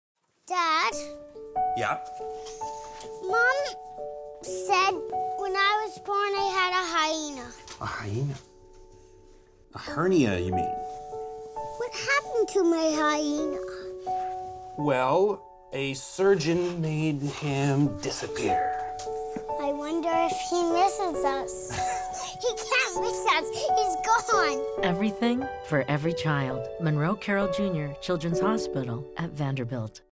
Everything for Every Child - surgery from the most common to the complex
When it's your child, no surgery is routine. Each family who comes to us is connected with  renowned surgeons in our surgery center designed to be your child's best opportunity for healing.

For more about surgery at Monroe Carell Jr. Children's Hospital at Vanderbilt,